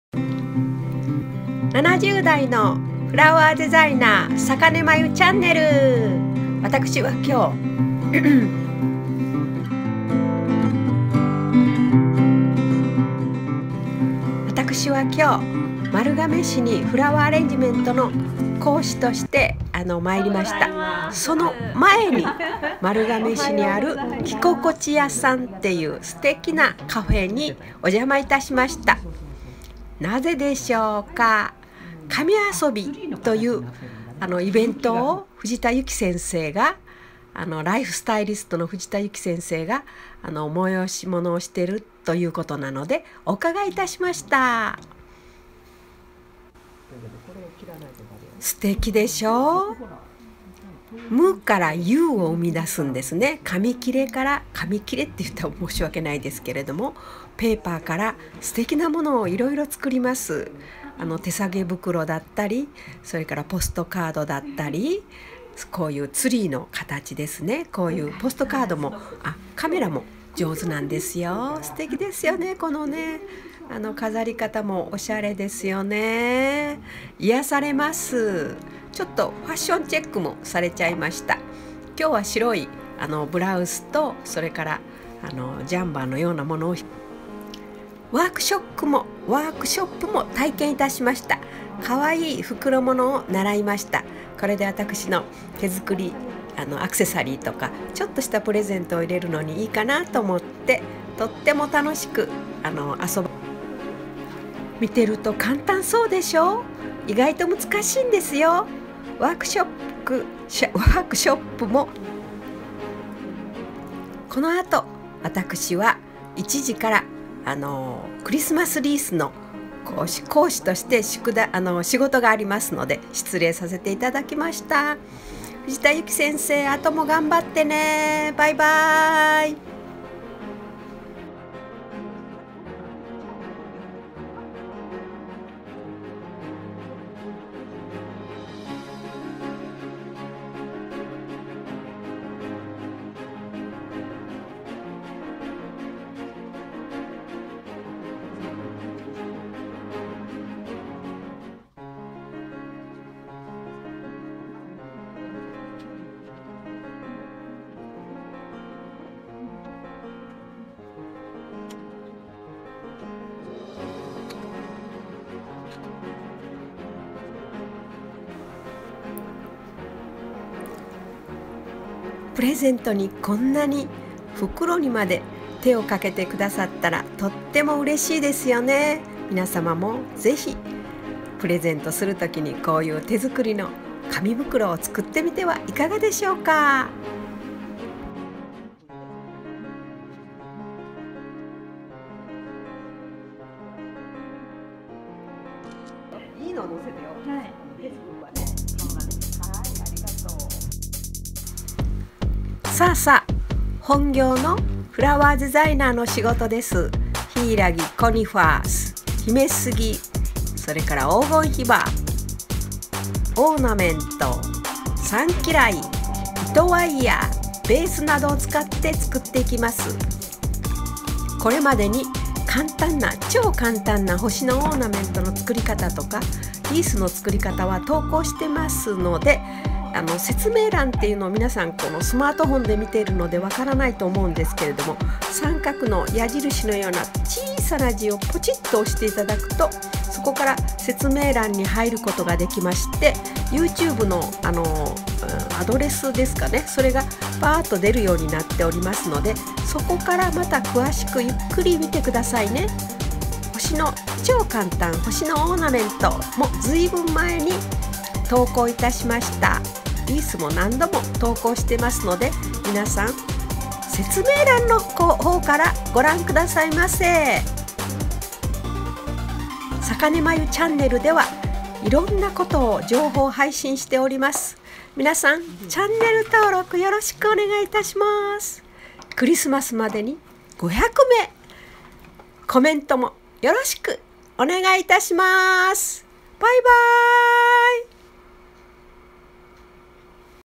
超愉しい xmas のフラワーアレンジメント。リース創りと、紙遊び❣️no240
ドイツ🇩🇪で習ったリースを、元に、ベースは、創ります

ヒイラギ
黄金ヒバ
コニファーフォレスト
ヒメスギ〈ヒムロスギ〉
サンキライ
糸ワイヤー28番

リースベース［ DAISO］
ガムテープ
グルーガン

阪根フラワースクール
香川県 坂出市